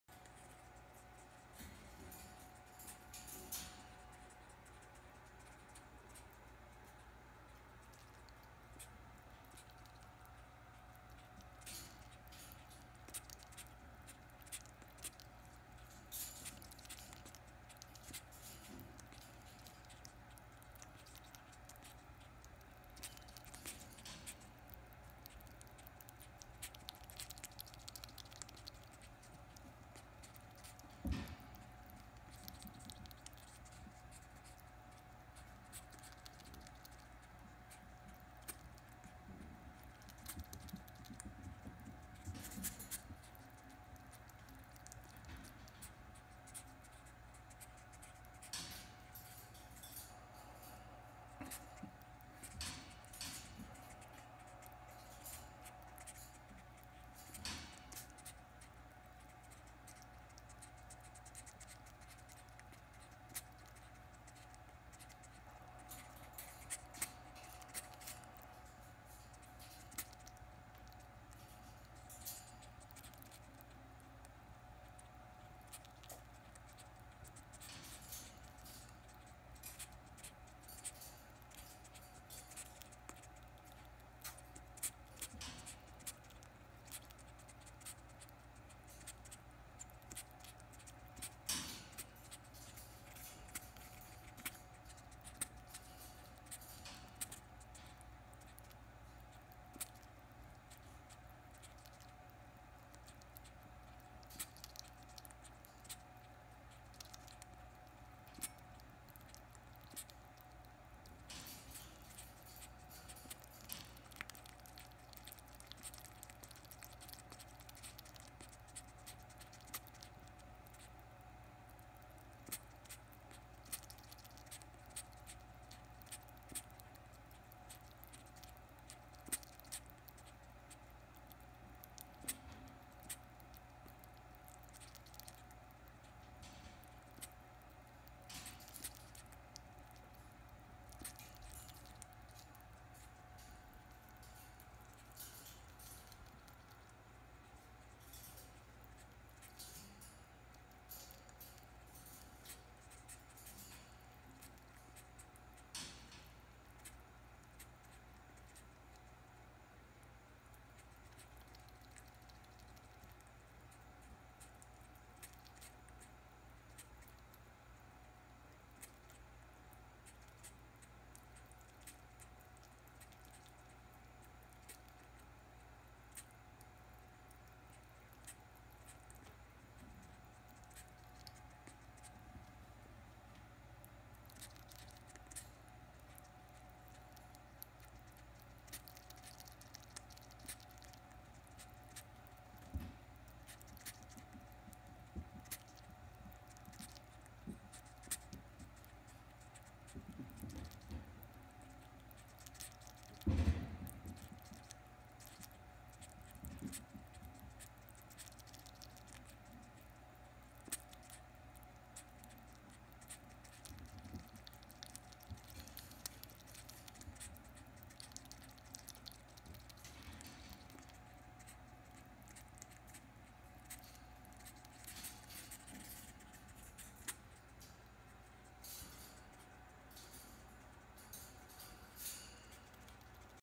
Sabrina Enjoying Her Avocado Treat (ASMR)
Despite not having upper incisors, Sabrina still enjoys avocado and many other foods. I'm thankful everyday that she's alive and pain-free.